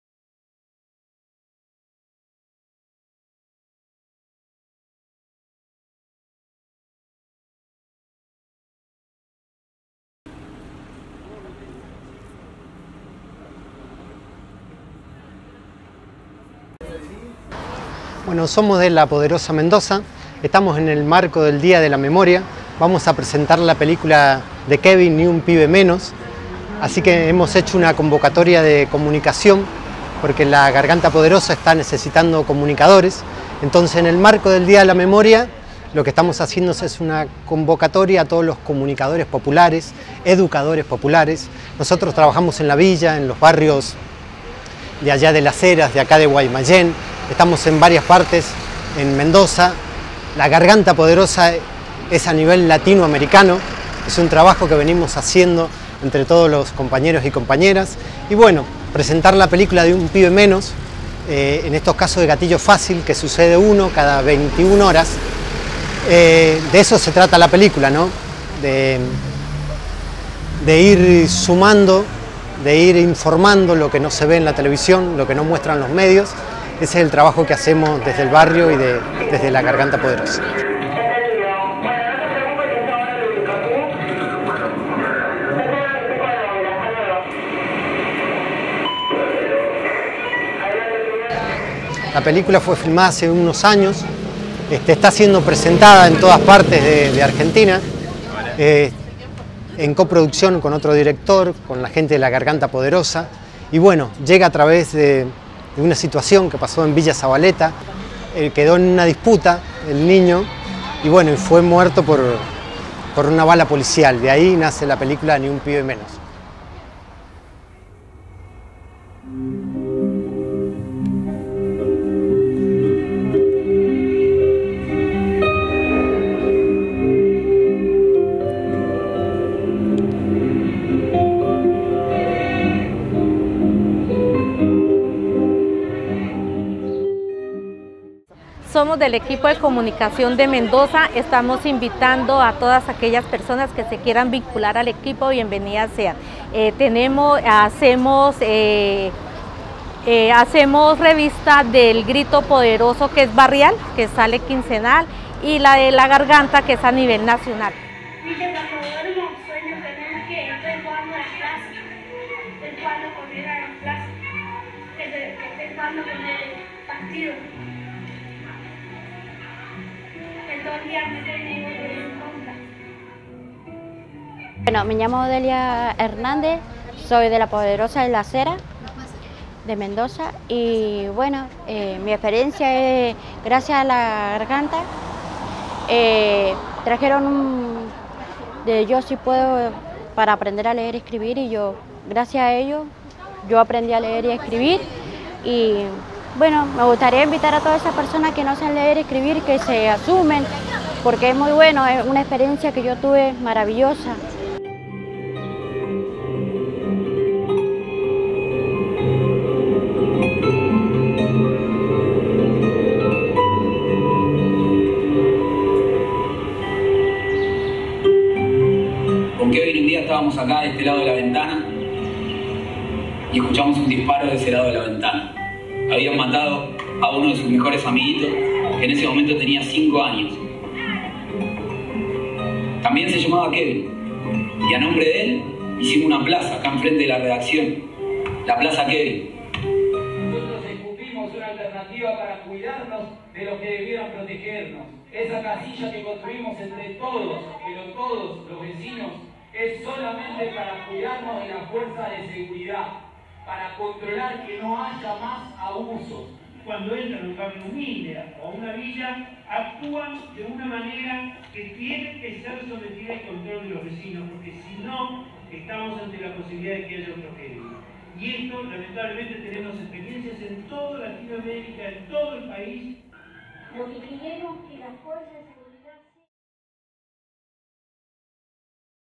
"Ni Un Pibe Menos", Presentación en Mendoza 23 de marzo de 2019
En el marco de la semana para la Memoria, la organización La Poderosa de Mendoza presentó el documental "Ni Un Pibe Menos" (2017) en el Centro Cultural Armando Tejada Gómez de Guaymallén. La película fue realizada a partir del asesinato de Kevin, un niño del barrio Zavaleta, Ciudad de Buenos Aires, quien fue alcanzado por una bala policial. Este es uno de los miles de casos de Gatillo Fácil en nuestro país, y el área de Comunicación de La Poderosa trabaja para visibilizarlos. Para más información compartimos su web